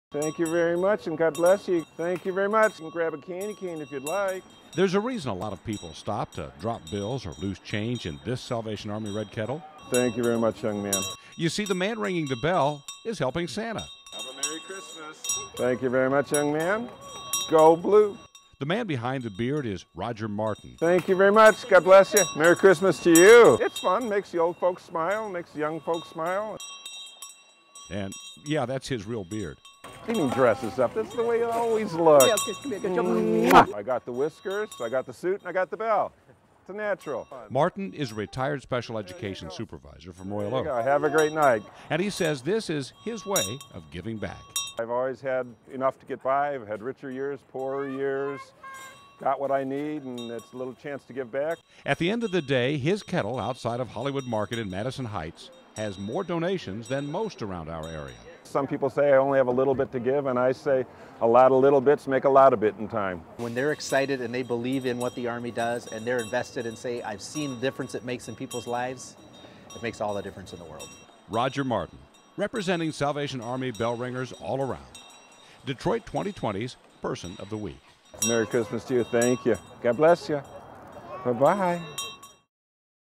Person of the Week--Roger Martin
Salvation Army bell ringer is Detroit 2020's Person of the Week.